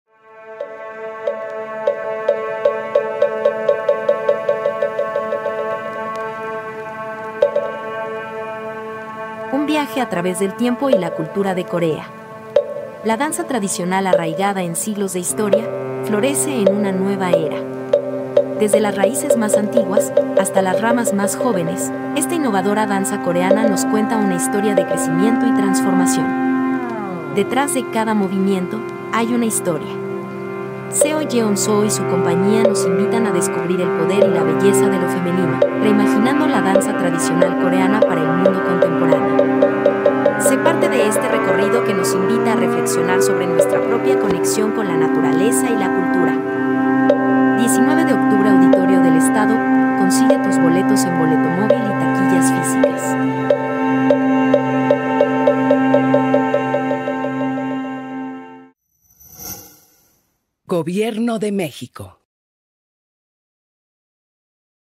🔹 𝙈𝙪𝙟𝙚𝙧 𝘿𝙖𝙣𝙘𝙚 𝘾𝙤𝙢𝙥𝙖𝙣𝙮 en el 52FIC
Disfruta de la pasión por la danza a través de esta vibrante presentación que nos acercará a la raíz de las artes de Corea.

 🗓️19 de octubre 20 h, 20 de octubre, 12 h
 📍 Auditorio del Estado
Consigue tus boletos en Boletomóvil y TaquillasFIC